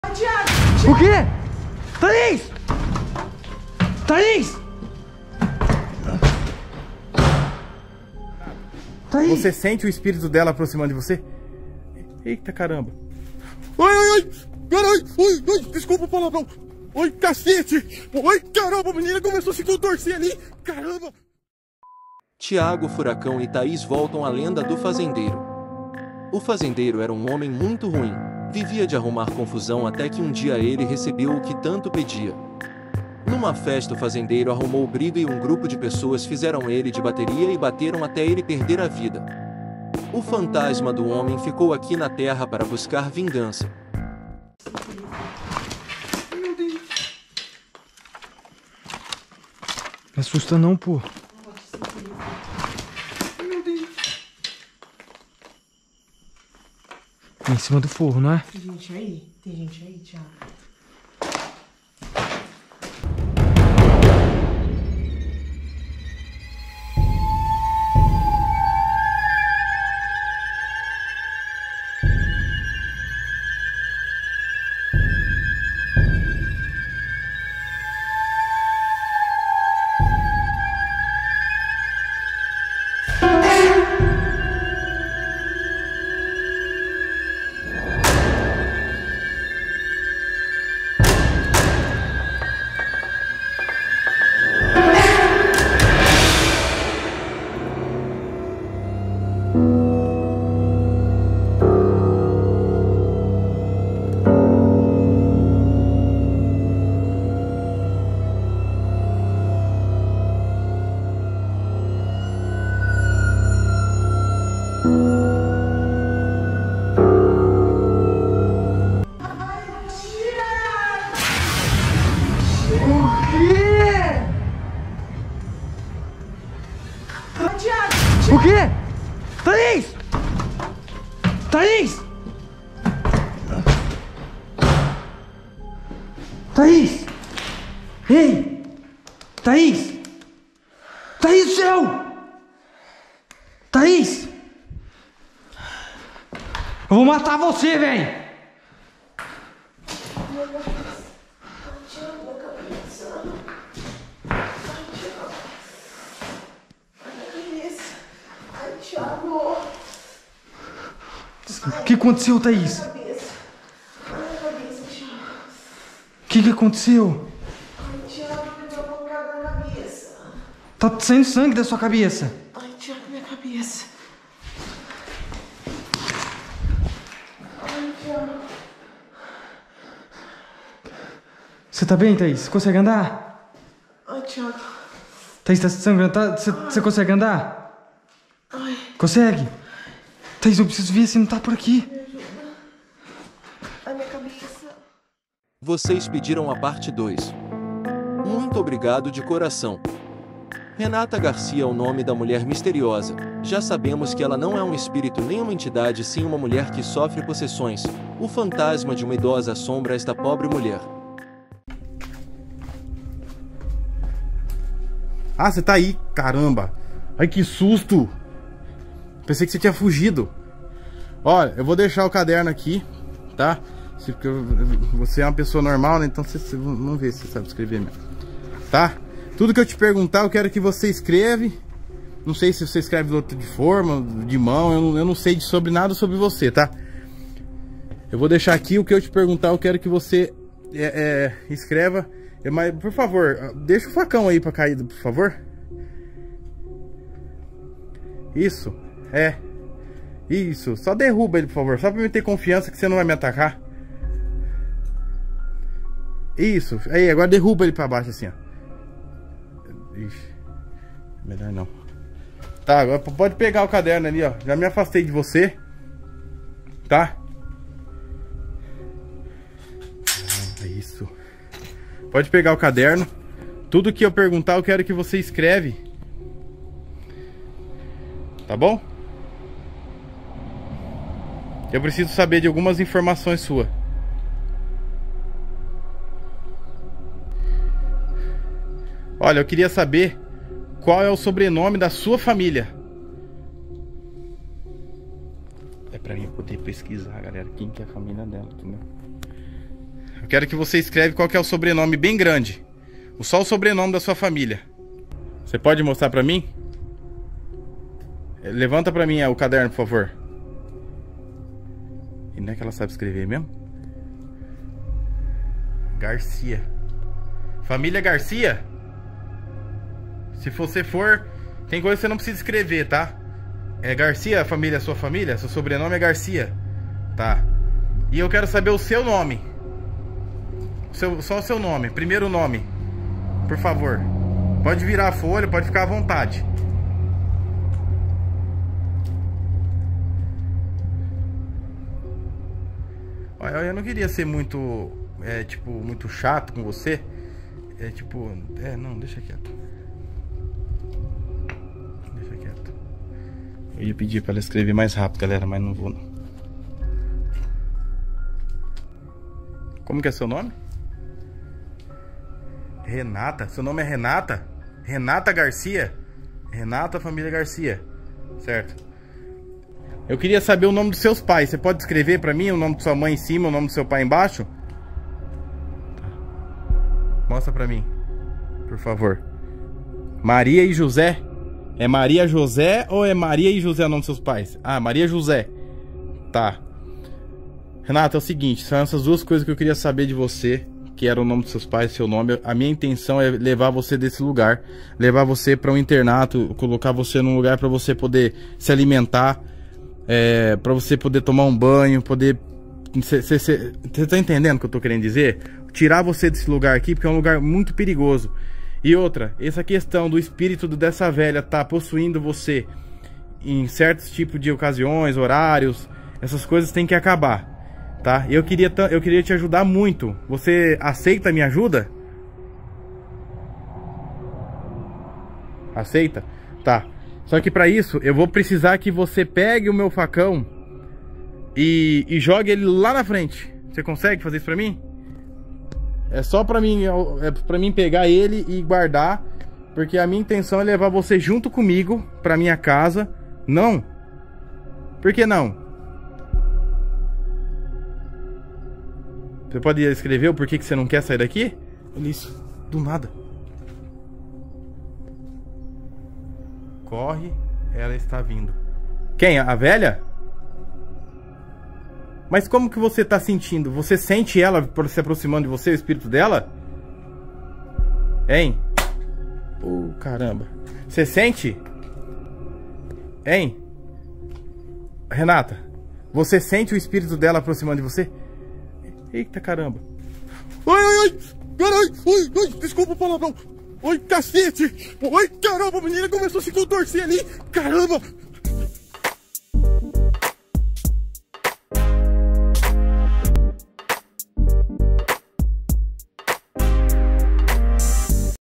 THIAGO FURACAO E THAIS FILMARAM O FANTASMA DO FAZENDEIRO E RENATA GARCIA SOFRE POSSESSÃO
Thiago furacao e thais foram até uma casa antiga casa abandonada com relatos de assombração do fantasma do fazendeiro. Hoje vamos continuar acompanhando o rodolfo augusto e a mulher misteriosa chamada renata Garcia.
Thiago furacao e thias voltam a lenda do fazendeiro. O fazendeiro era um homem muito ruim, vivia de arrumar confusão até que um dia ele recebeu o que tanto pedia. Numa festa o fazendeiro arrumou briga e um grupo de pessoas fizeram ele de bateria até ele perder a vida.
vocês pediram a parte 2. Muito obrigado de coração. Renata Garcia é o nome da mulher misteriosa, já sabemos que ela não é um espírito nem uma entidade e sim uma mulher que sofre possessões, o fantasma de uma idosa assombra esta pobre mulher.

vídeos assustadores de fantasmas pra dar boas risadas e tomar muitos sustos

meu INSTA: tl.jackson.tl